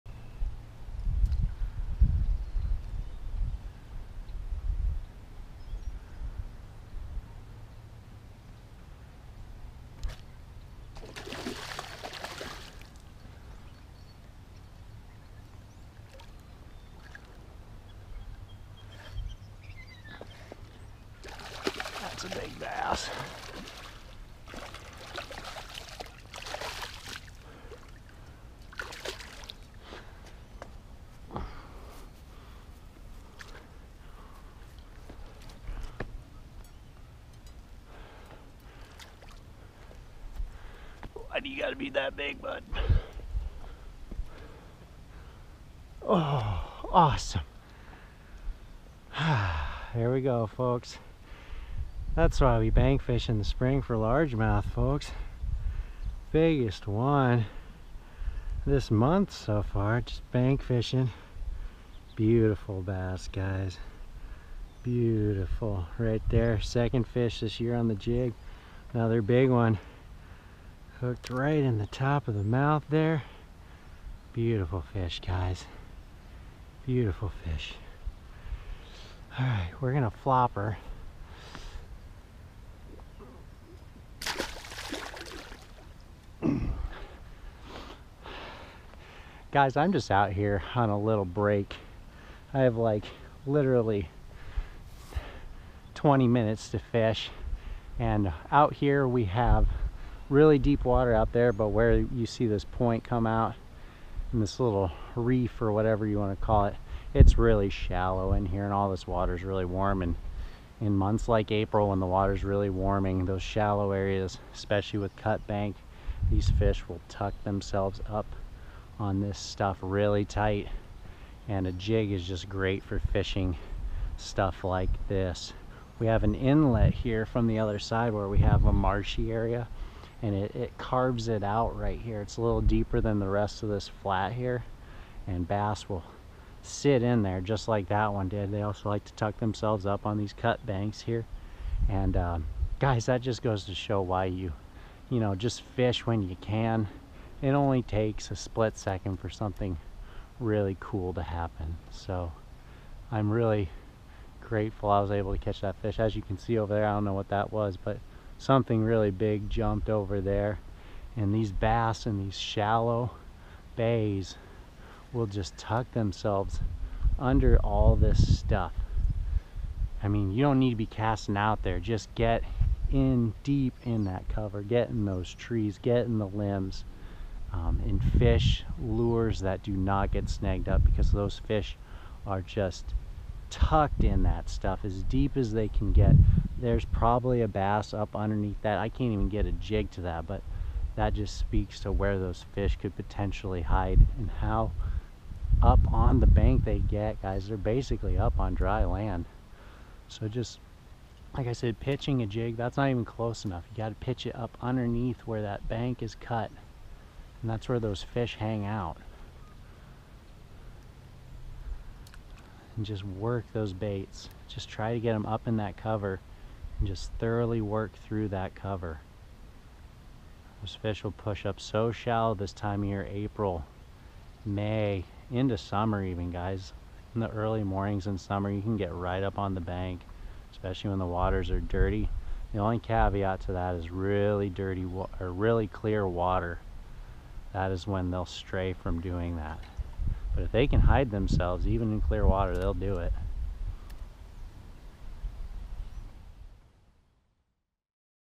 Catch Spring Bass ONLY FEET AWAY With A Jig -Iowa Bass Fishing
We are doing some quick Iowa Bass fishing from the bank to demonstrate the effectiveness of a Jig and Trailer when water temperatures start to go above 50 degrees. We also discuss fishing the Pepper Custom Baits Micro Jig with a Berkley Chigger Craw trailer and, more importantly, WHAT KIND OF COVER TO LOOK FOR!! Now, go catch yourself some Pre-spawn bass folks! Enjoy and happy fishing spring 2024!

0:00 Pitching A Jig To A Bass
0:53 Bass Caught
1:37 Where to Fish A Jig